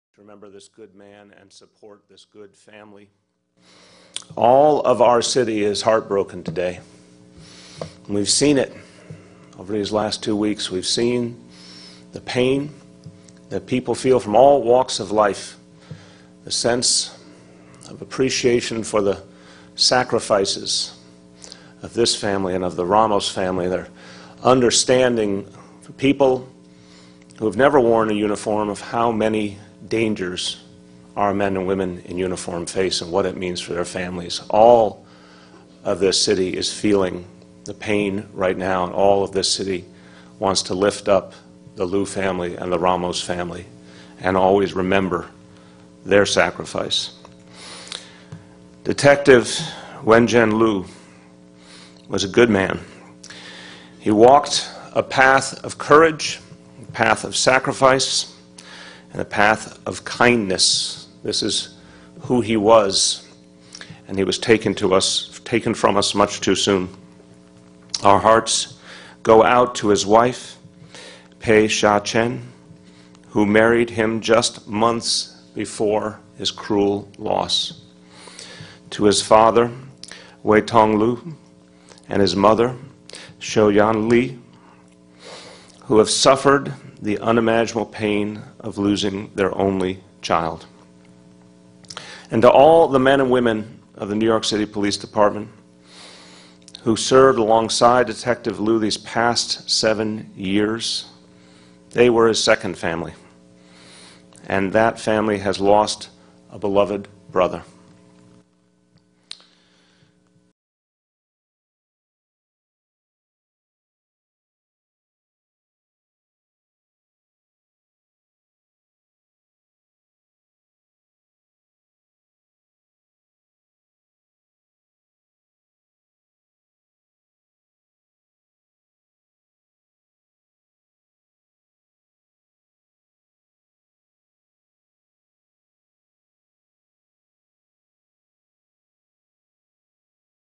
De Blasio speaks at funeral for police officer Wenjian Liu - Daily Mail
Thousands of police officers are paying their respect to a New York Police Department officer shot to death along with his partner, Officer Rafael Ramos. Buddhist monks led a Chinese ceremony for Officer Liu, followed by a traditional police ceremony with eulogies led by a chaplain. Liu, 32, had served as a policeman for seven years and was married just two months when he was killed with his partner, Officer Rafael Ramos, on December 20. Ahead of the final rites on Saturday, police commissioner William Bratton warned his officers to refrain from any 'act of disrespect' toward Mayor de Blasio.